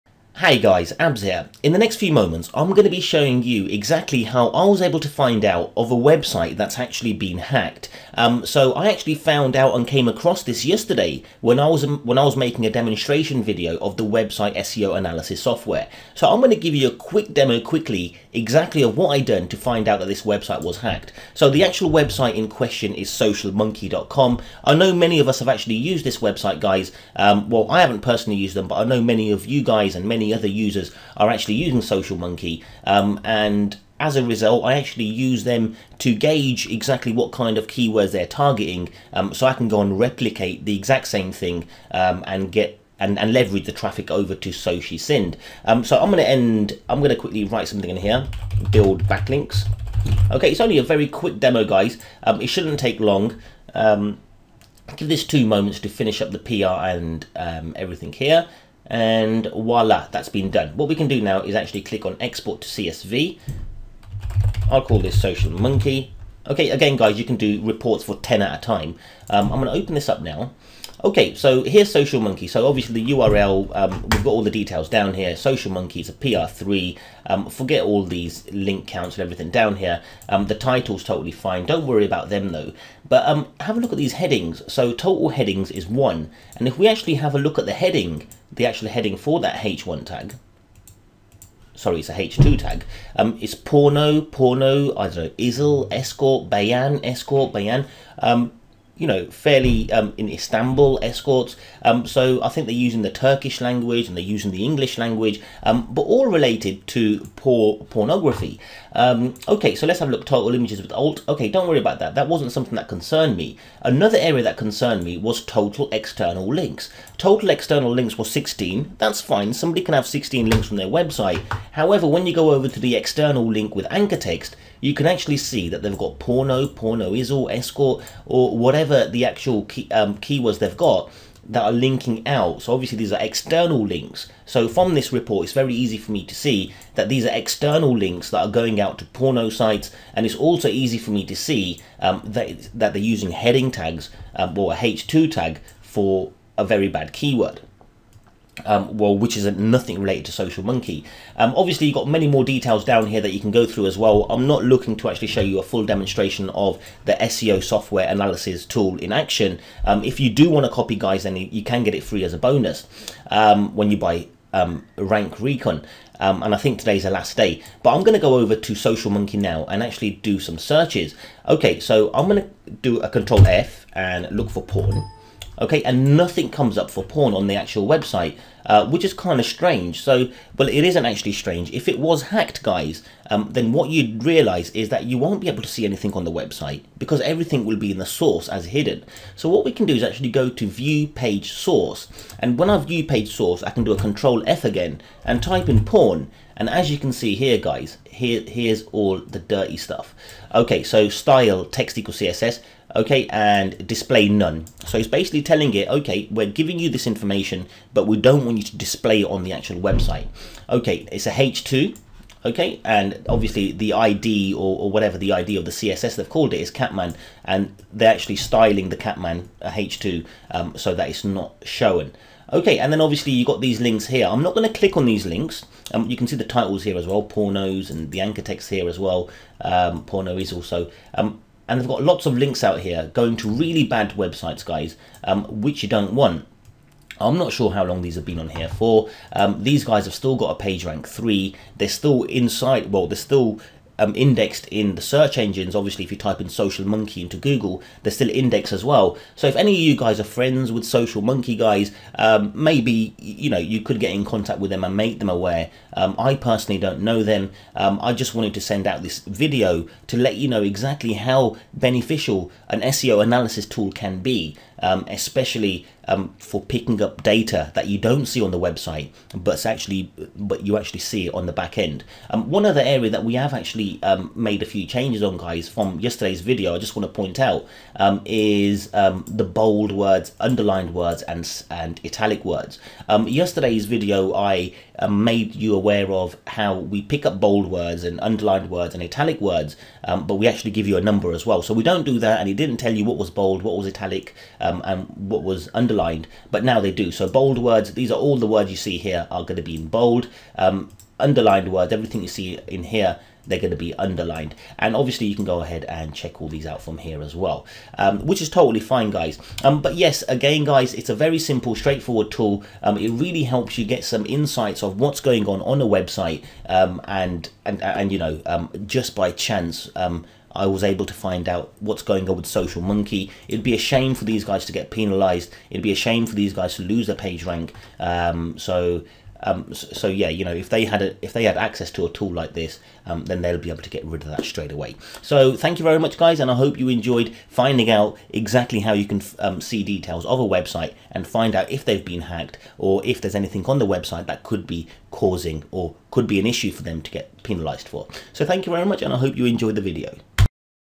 SocialMonkee Hacked - See my seo tool pick up on this
seo analyses software picks up on porn words on socialmonkee